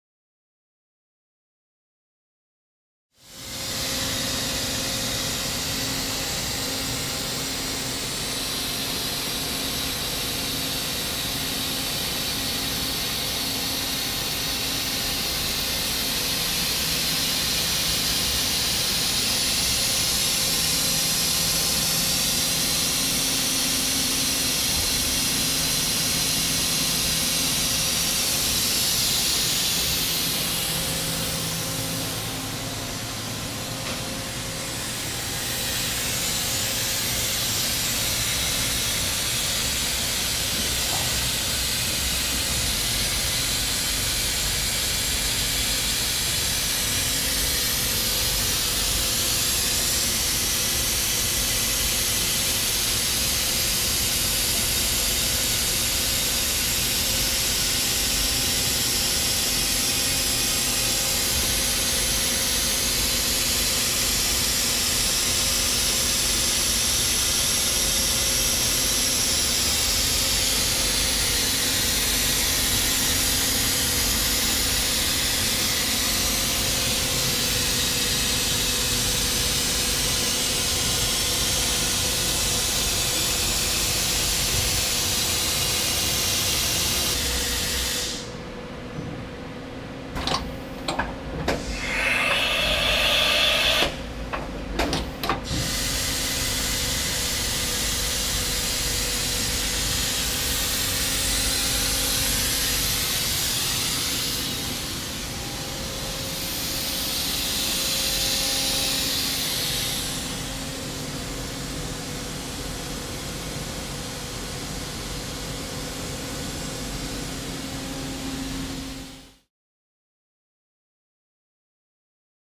2001 KASUGA QUANTUM 1000V VMC
Machine Dims
Length - 119"
Depth - 112.5'
Height - 115"
Weight - 18,632

Coolant Tank
72" x 33" x 27"
500 lbs

Chip Conveyor
179" x  28" x 64"
1,500lbs

Pickup address:
5451 Commercial Drive
Huntington Beach, Ca
92649